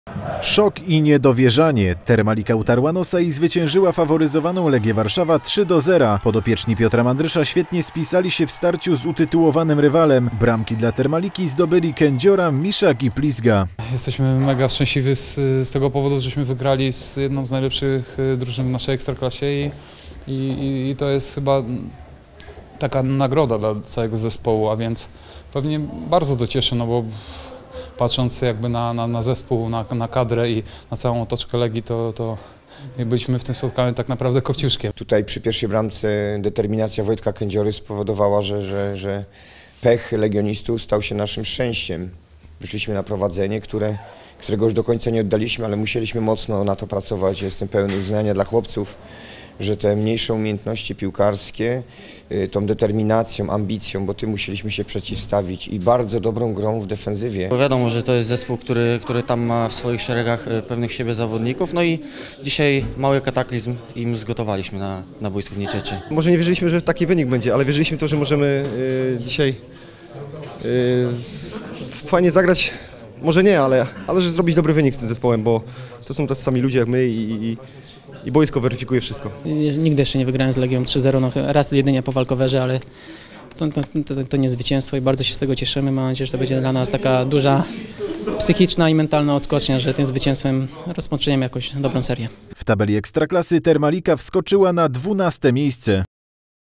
Termalika utarła nosa Legii! Historyczne zwycięstwo "Słoni"
Szok i niedowierzanie. Termalica utarła nosa i zwyciężyła faworyzowaną Legie Warszawa 3:0. Podopieczni Piotra Mandrysza świetnie spisali się w starciu z utytułowanym rywalem. Bramki dla Termaliki zdobyli Kędziora, Miśak i Plizga

W tabeli Termalica wskoczyła na 12 miejsce.